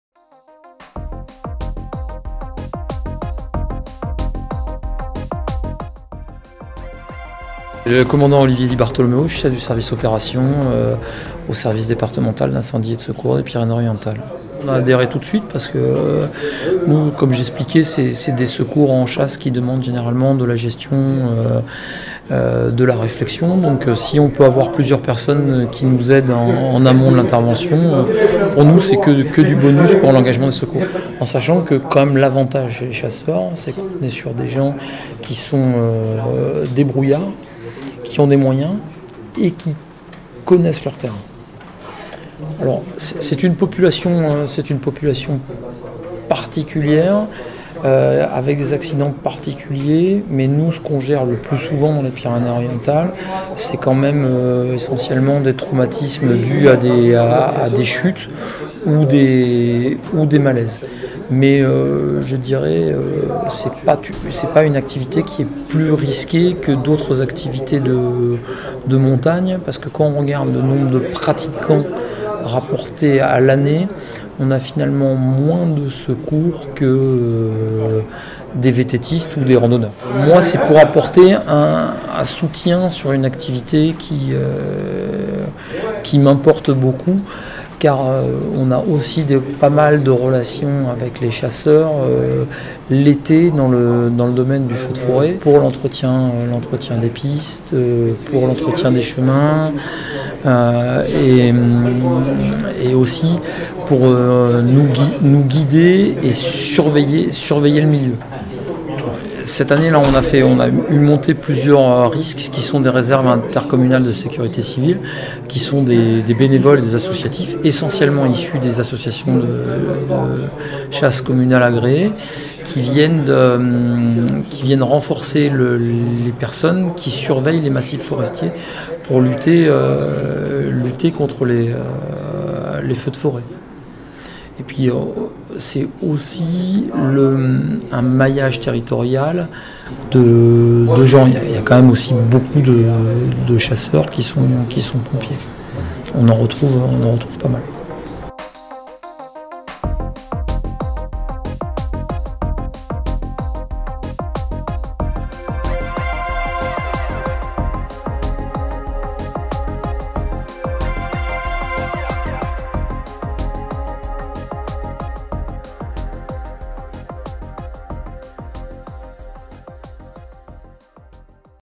Formation à la Sécurite des chasseurs des Pyrénées-Orientales.
ITW du Commandant Olivier di Bartolomeo, Chef du Service Opération du SDIS66 des Pyrénées-Orientales sur la coopération en matière de formation des chasseurs avec la FDC66